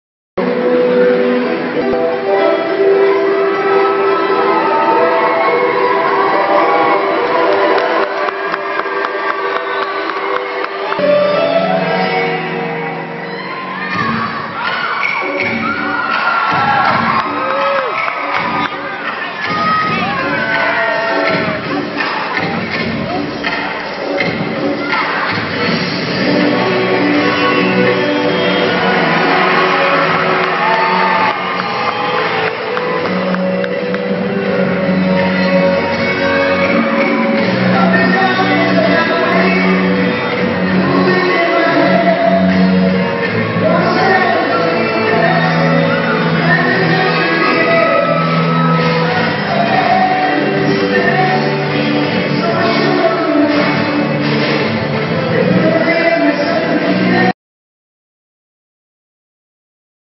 The CS Fullerton dance team performs at halftime of the basketball game against UC Riverside.
By: Mike Sprague - Reporter for the Whittier Daily News covering La Mirada, La Habra Heights, Montebello, Santa Fe Springs, water issues, politics